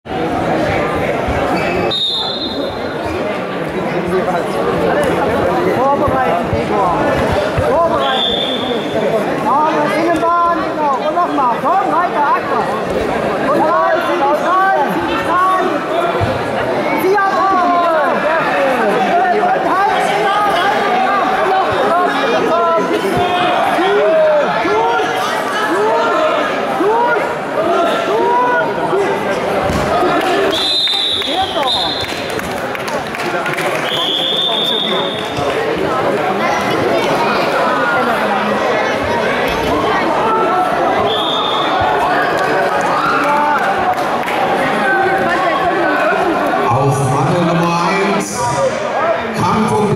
2. Runde B-Jugend vs. C-Jugend Rashid Hamo (TSG Halle-Neustadt) - Gregor Wolf (SV Halle) 31kg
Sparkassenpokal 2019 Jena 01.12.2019
Freundschaftskampf zwischen Rashid Hamo (B-Jugend) und Gregor Wolf (C-Jugend) in 31 kg. Der ältere Hamo hatte in seiner Alters- und Gewichtsklasse keinen Gegner und forderte den jüngeren Gregor Wolf vom SV Halle zu einem Freundschaftskampf heraus.